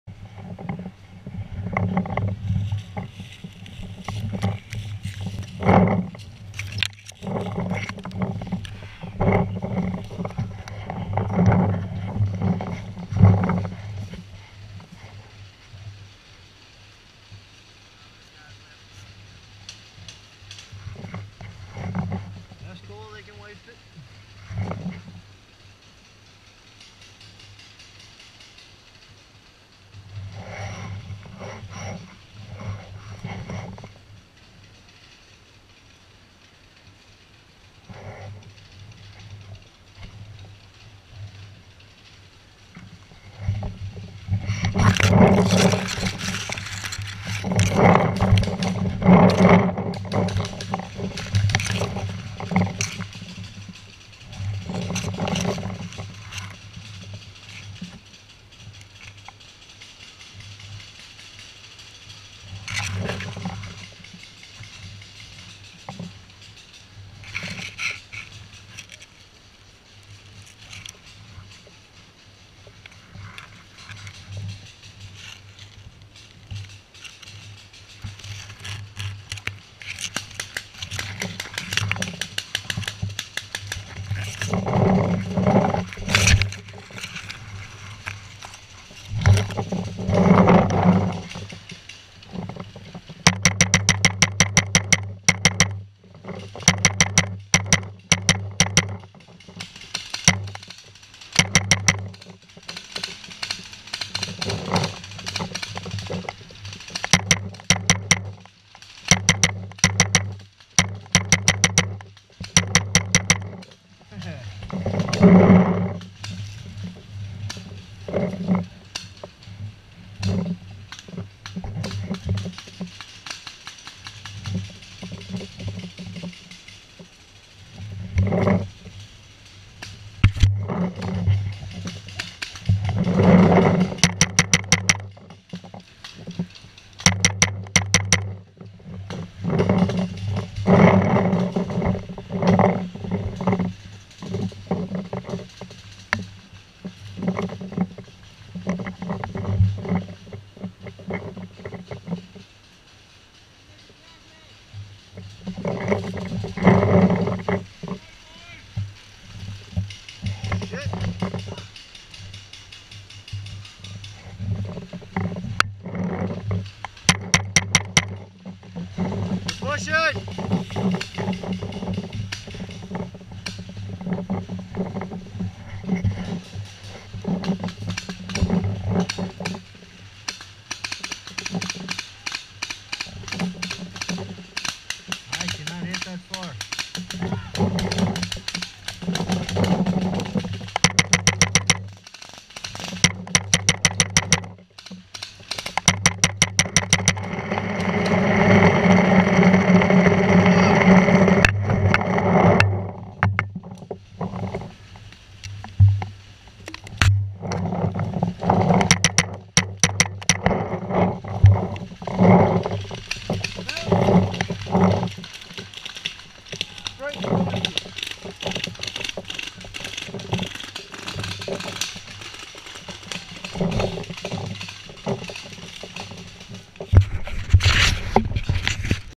2014 D-Day Utah beach
The 6 FJR held the hill above Utah beach against the horde of the 101st Airborne. They came in hard and fast and it was great. We held the top of the hill until the refs pushed us back down it about halfway with the quote of "Your're killing them too fast."

I'm using an Axe.